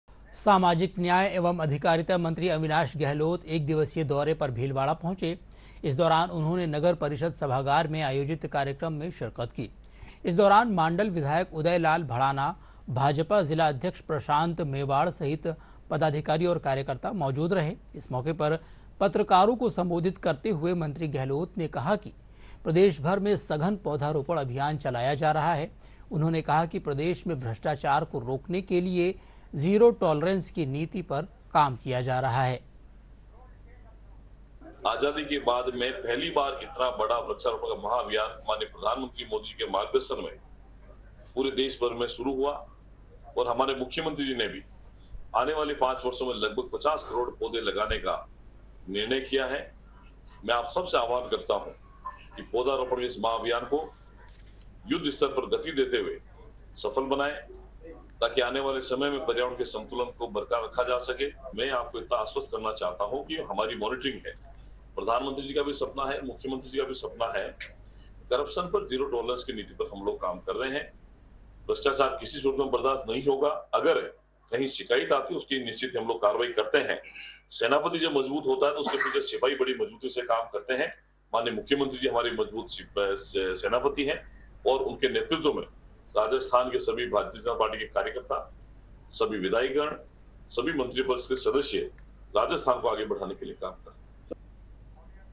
मंत्री अविनाश गहलोत का भीलवाड़ा दौरा, बोले- 'सेनापति मजबूत है इसलिए सिपाही भी...'
राजस्थान सरकार में सामाजिक न्याय और अधिकारिता मंत्री अविनाश गहलोत के भीलवाड़ा आगमन पर स्थानीय सर्किट हाउस में बीजेपी जिलाध्यक्ष प्रशांत मेवाड़ा के नेतृत्व एवं विधायक उदयलाल भडाना के सान्निध्य में भव्य स्वागत अभिनंदन किया गया. कैबिनेट मंत्री अविनाश गहलोत ने इस अवसर पर मीडिया से बातचीत करते हुए कहा कि राजस्थान के मुख्यमंत्री भजनलाल शर्मा सेनापति के रूप में मजबूती से कार्य कर रहे हैं, इसलिए उनके सिपाही सभी मंत्री और विधायक भी उसी मजबूती से आमजन, गरीब, युवा, महिला, किसान के साथ प्रत्येक वर्ग के कल्याण के संकल्प के साथ कदम से कदम मिला रहे हैं.